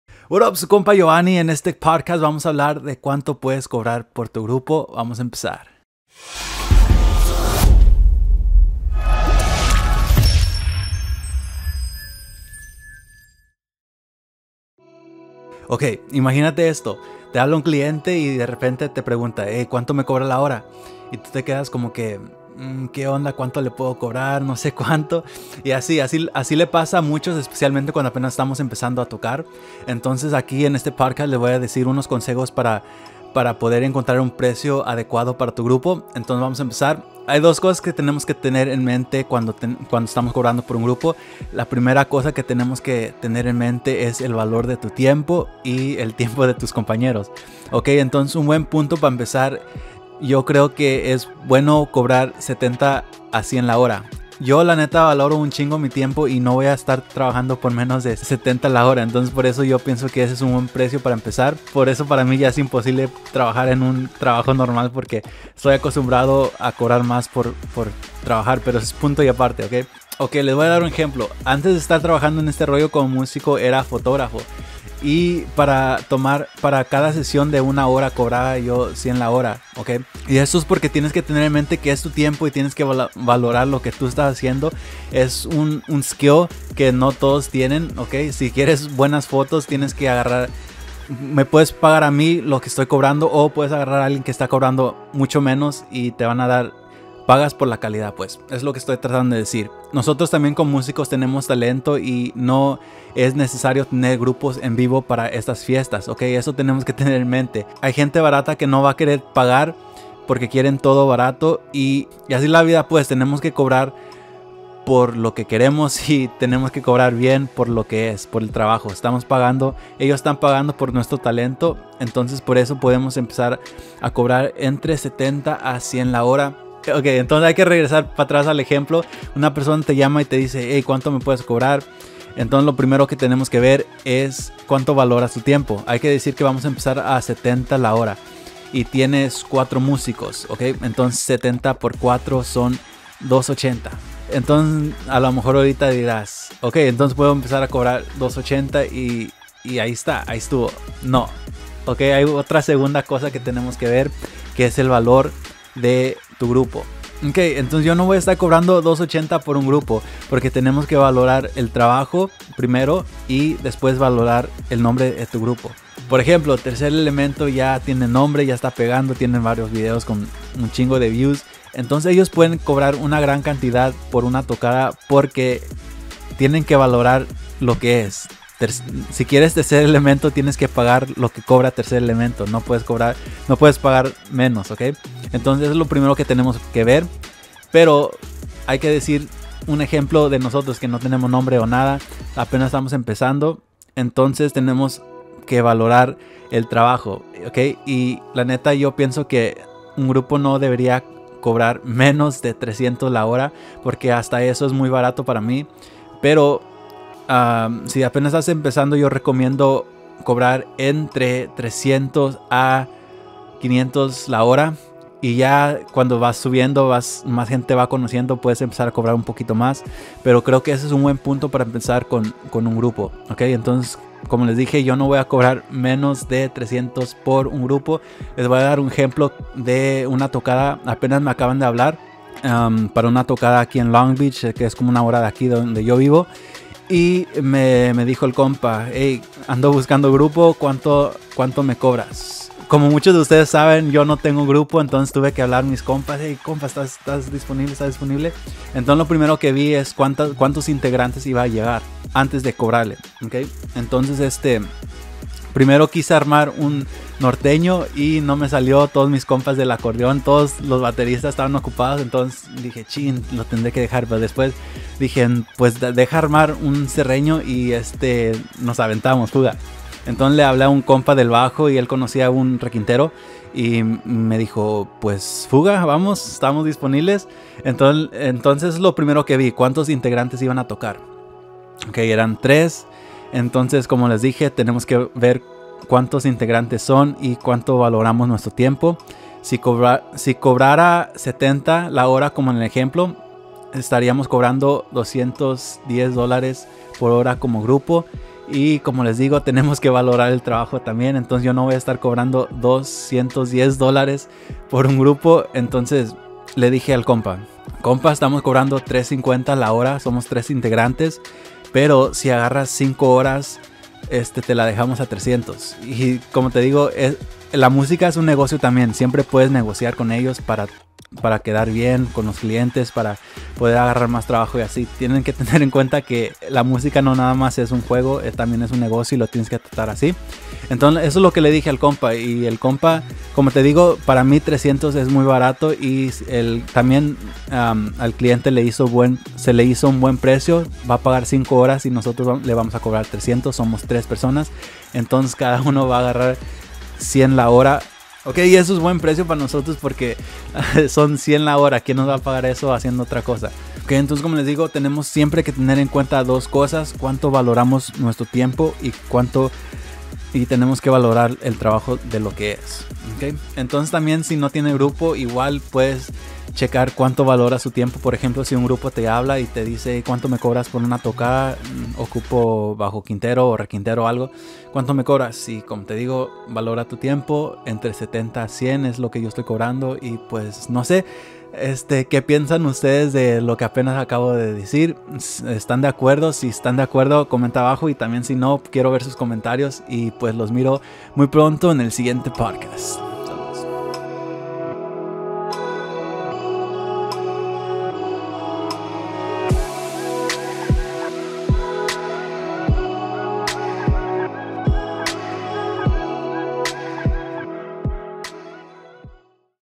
Cuanto Puedo Cobrar Por Tocada? | Podcast
What up Requintazo Squad!

El otro dia alguien me pregunto que es un buen precio pa cobrar pa su grupo.

Ay dos cosas que tenemos que tener en mente cuando vamos a cobrar. La primer cosa es cuanto valoramos nuestro tiempo. La neta yo valoro mi tiempo y no voy a trabajar por menos de 70 la hora. Les recomiendo que hagan lo mismo!

La segunda cosa que tenemos que tener en mente es que tenemos que valorar el trabajo y el grupo. Nosotros como musicos tenemos un talento que muchas personas no tienen. La musica en vivo es un priviligio no una necesidad.

Tenemos que valorar nuestro trabajo y  cobrar bien por lo que es. Va ver gente que no quiera pagar pero ni modo. Asi es la vida. Yo puedo querer un carro pero si no tengo el dinero no me lo van a dar jajaja. Busca otros clientes que si te van a pagar lo que mereces.

Aqui abajo te dejo el video donde hablo mas de este tema. Espero te ayude. Deja un comentario con tu opinion para que lo pueda leer.

Te miro en el siguiente video!

Estas solo a un paso de alcanzar tus metas...

-Jovanny Castillo
ComoTocarChingon.com